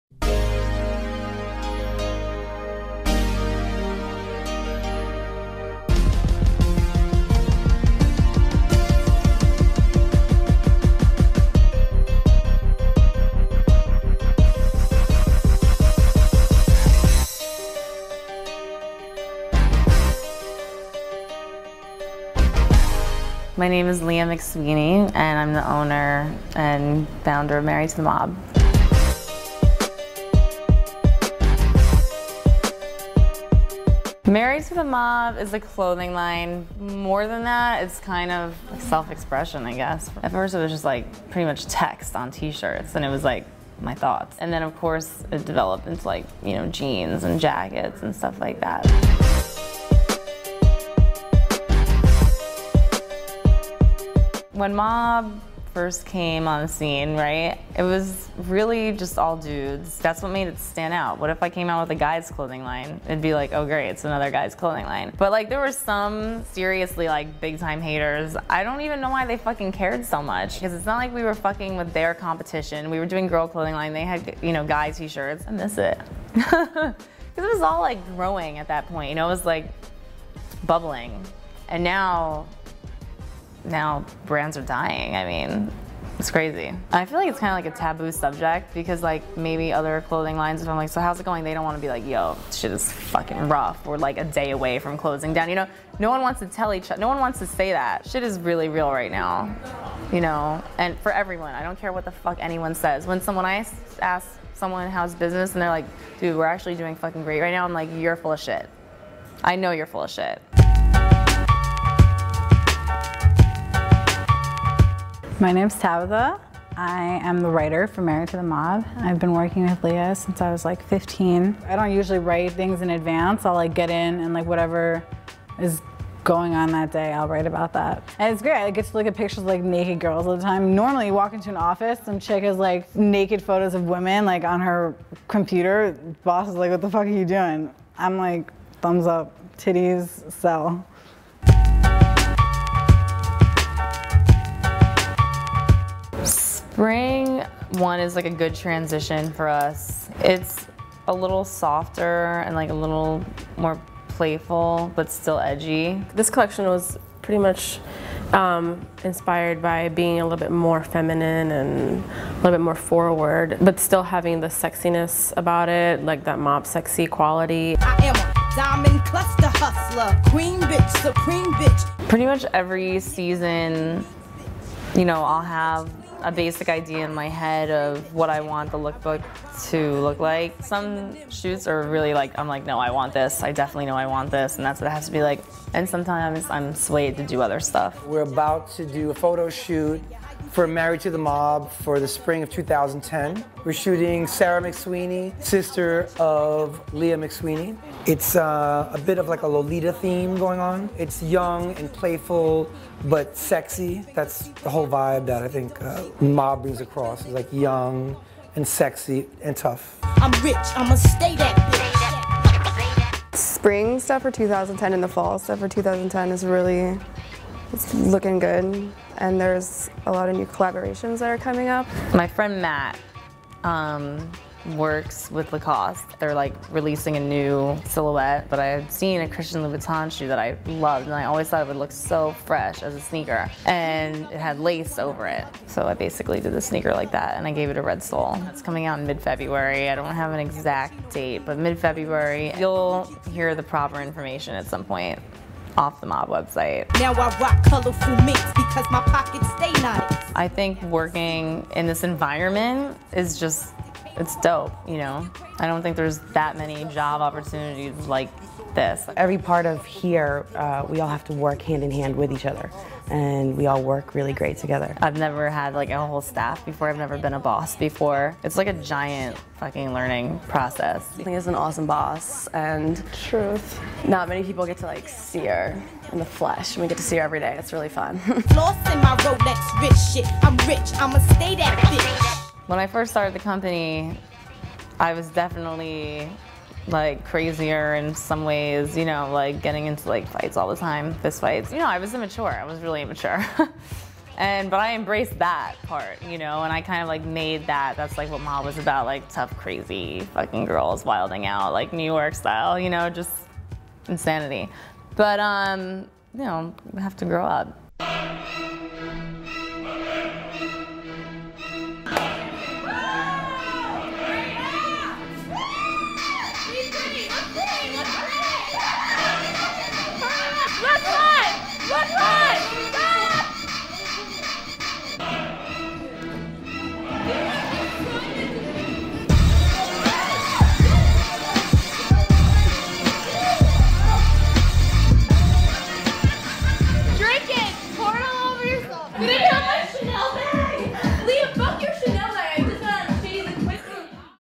HBTV Leah McSweeney: Married to the Mob & The Most Official Bitches
One of streetwear's few women labels, we visit the offices of Leah McSweeney and her MOB Girls. A brand with an attitude unlike any other, we're taken on a candid behind-the-scenes of Married to the Mob's office setting with a look into the various personalities that give MTTM its character. Along the way, Leah McSweeney and her crew talk about inspirations and the evolution of the brand.

Director: Kellen Dengler